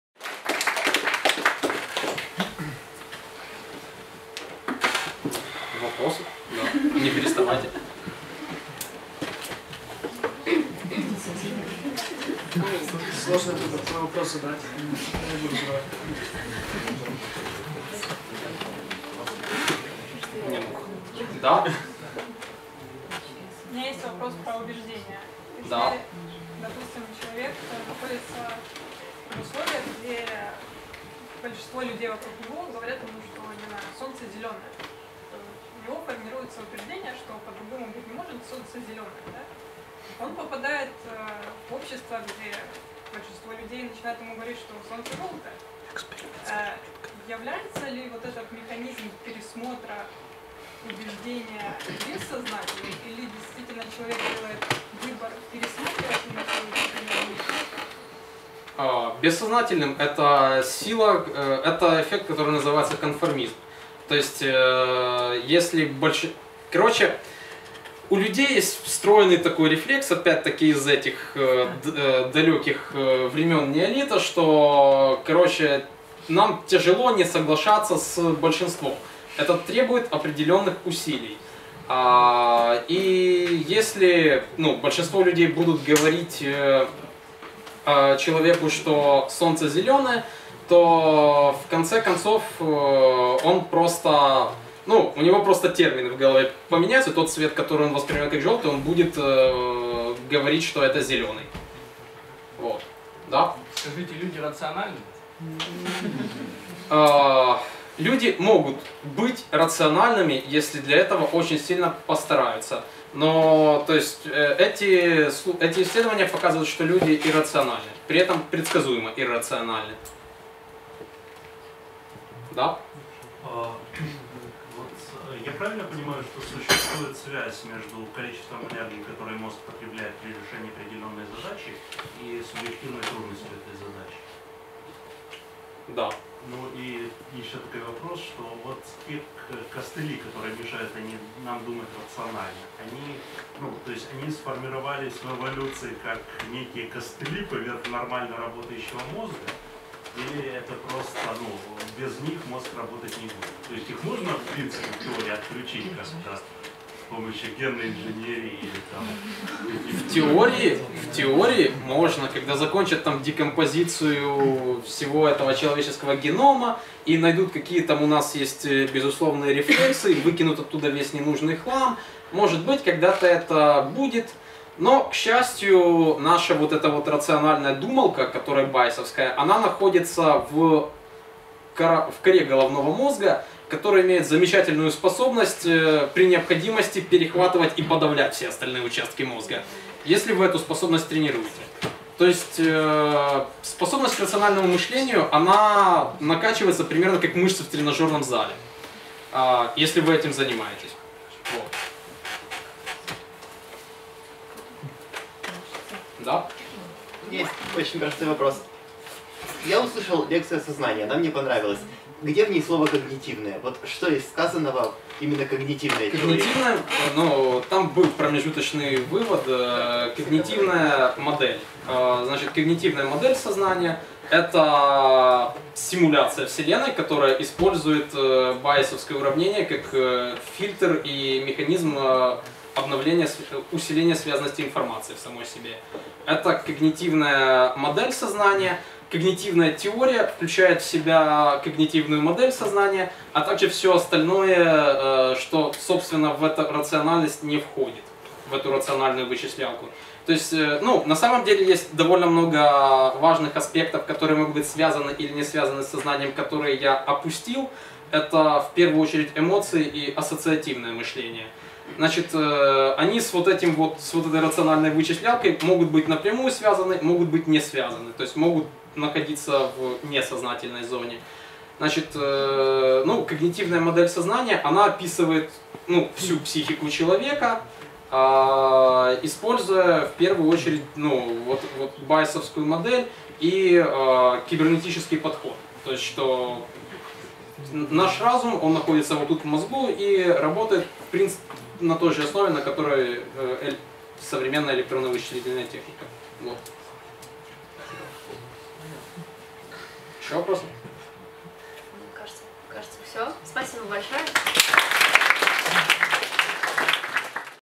15х4 - 15 минут о когнитивной теории сознания. Обсуждение
"Почему людям легко видеть вещи вокруг, а компьютерам очень сложно? Насколько наши решения — это действительно наши решения? Как работает наше сознание, как оно не работает и как именно это мешает нам жить?" - Дима Веселов

15х4 - это сообщество молодых ученых и фанатов науки.
Мы хотим, чтобы люди выступали и делились знаниями.